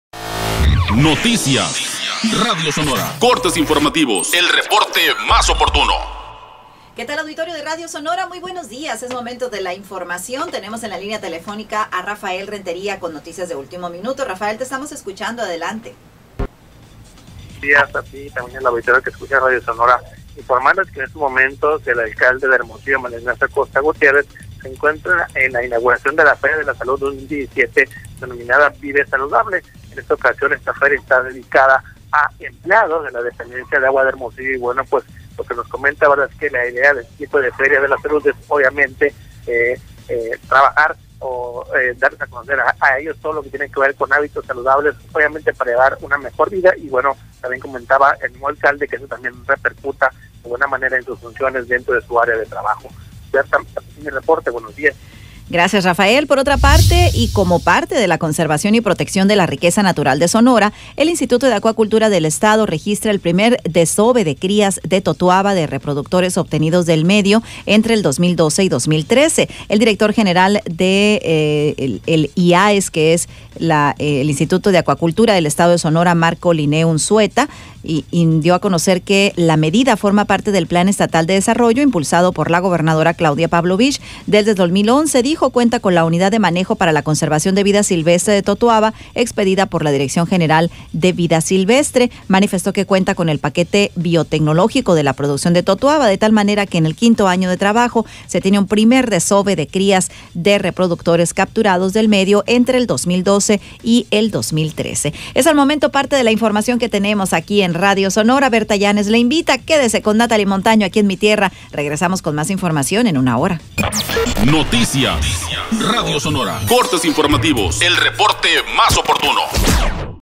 Cortes Informativos - 30 de mayo 2017 (Primero)
Se lleva a cabo la feria de la salud ‘Vive Saludable’ para personal de Agua de Hermosillo​. Aquí Bertha con los detalles.